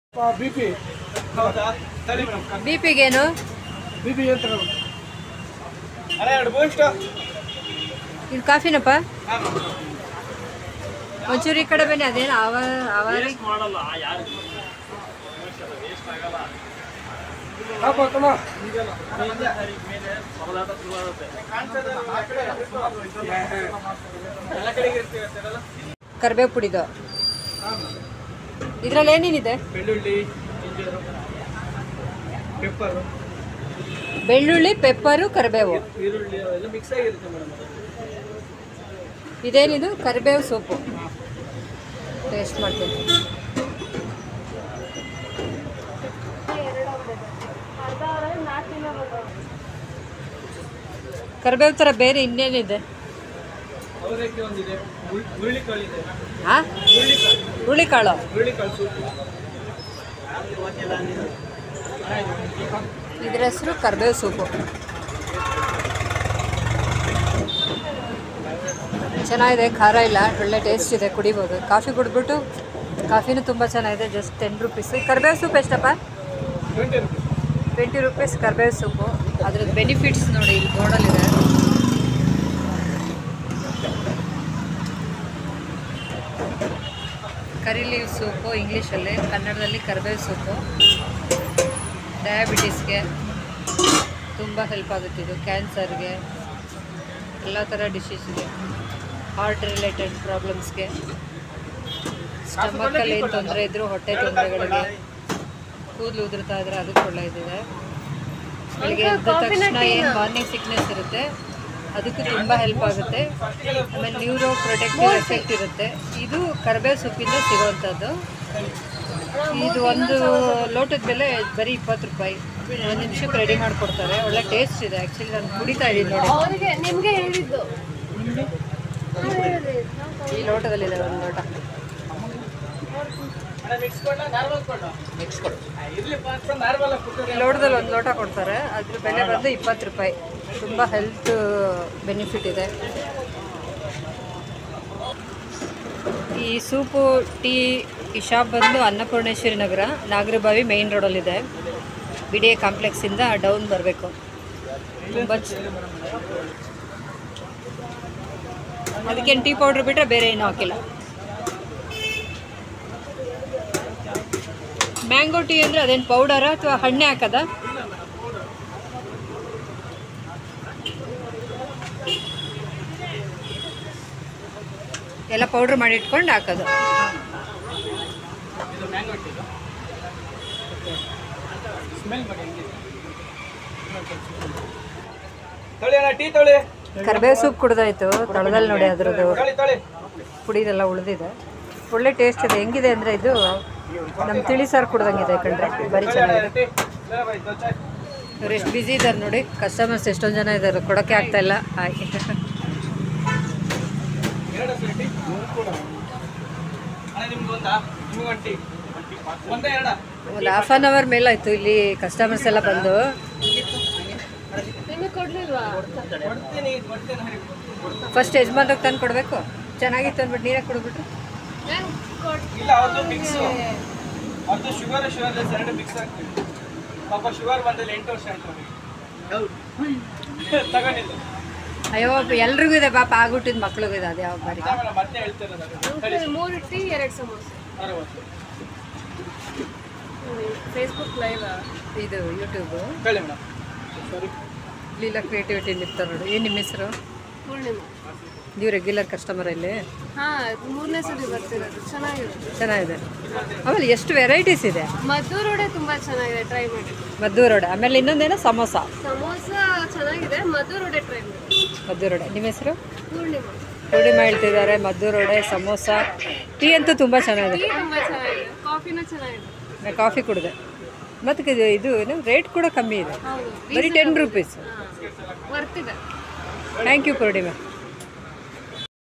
aavarike soup_20rs.karibevu soup_20.coffee 10rs.tea-10rs.wonderfull taste. nagara bahavi. Annapurnes
wari Nagar. bangalore.